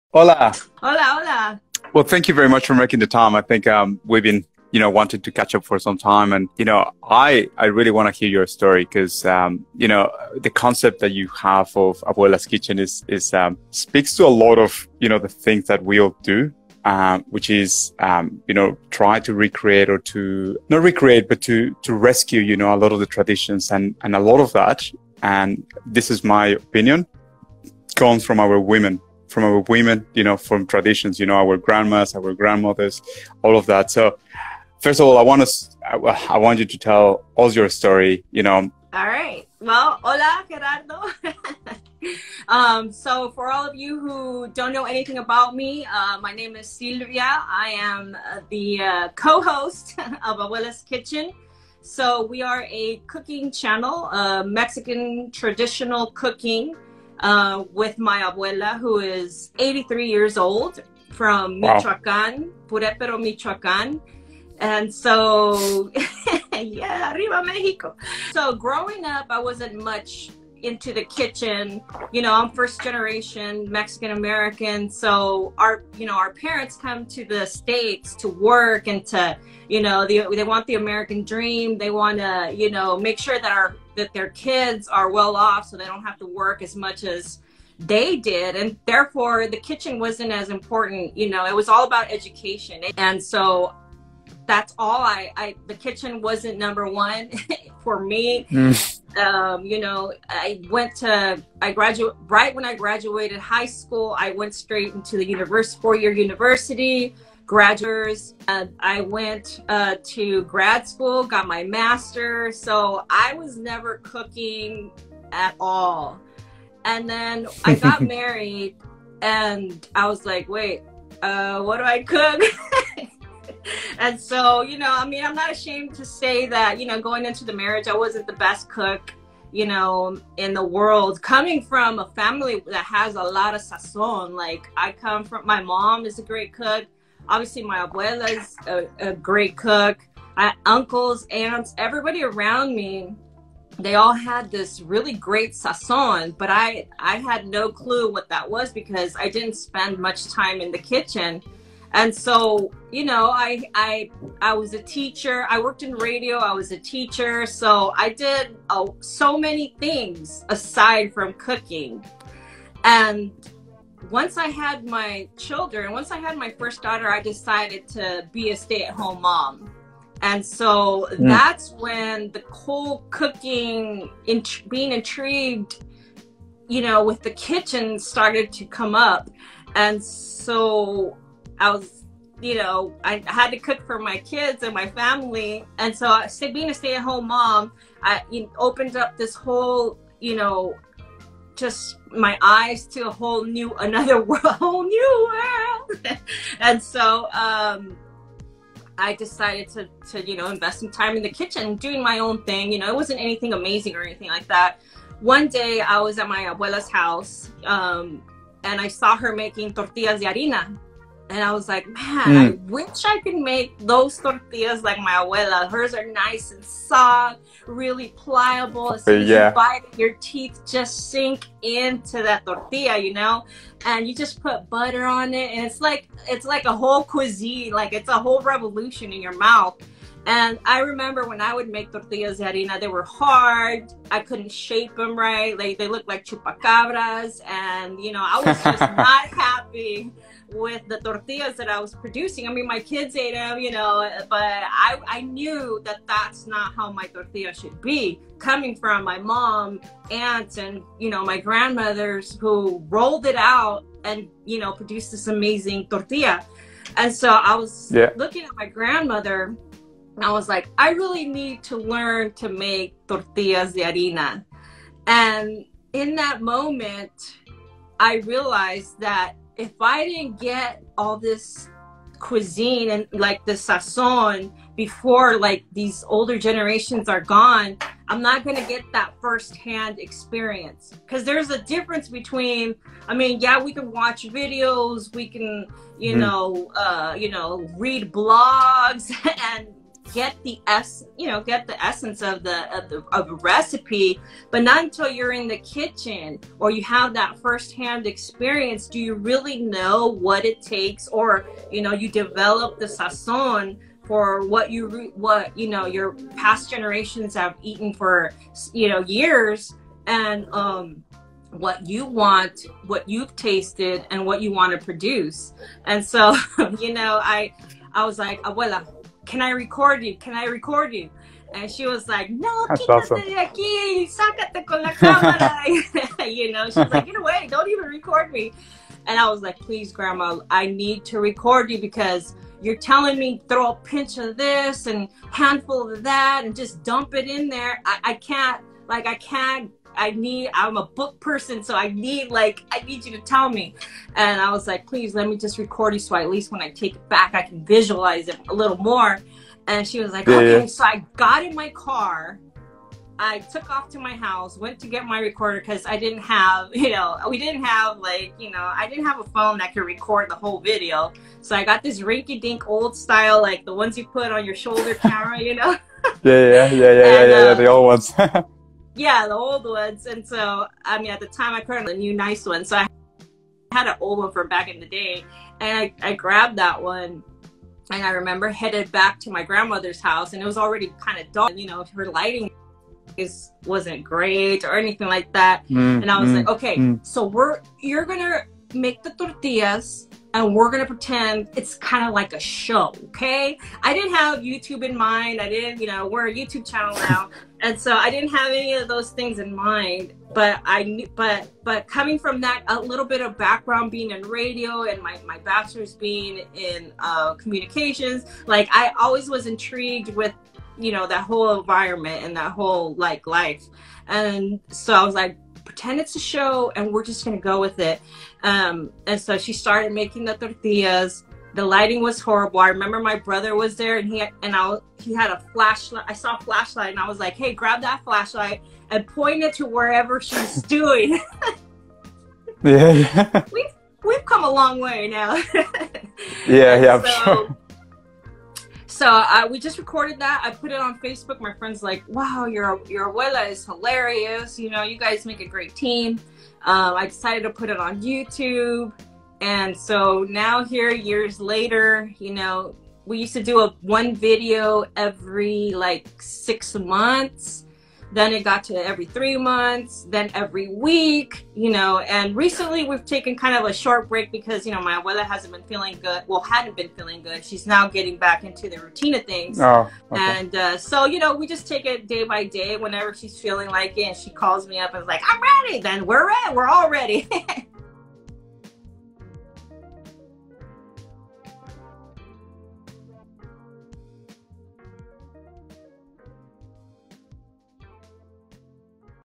What is Abuela's Kitchen? Interview with Silvia Co-Host of Abuela's Kitchen
La Cocina de la Abuela / Grandma's kitchen

it is all about stories, recipes and generational connections. Thank you Sylvia from @abuelaskitchen for such a warm and uplifting conversation 🧡